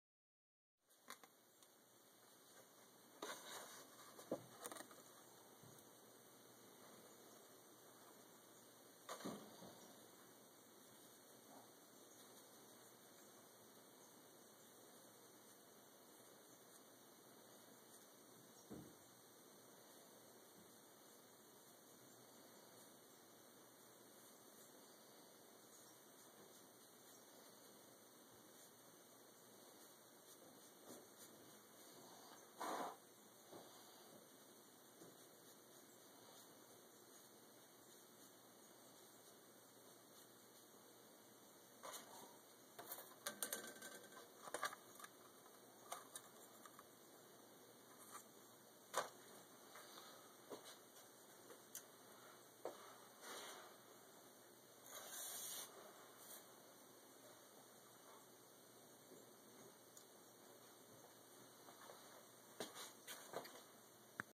Cadence sand shading 2
This is raw video footage presented for the client to view the progress of their custom order. This video will be edited and included in the final DVD given to them at delivery.